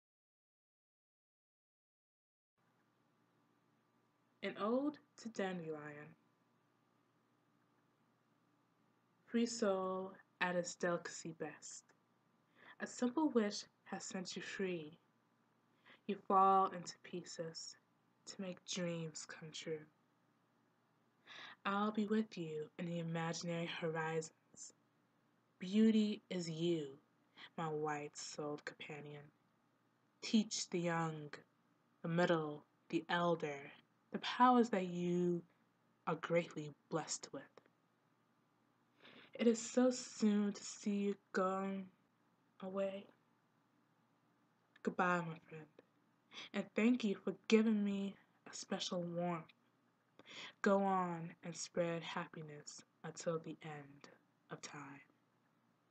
An Ode to Dandelion- A poem
An original short poem about the simple beauty of a white dandelion.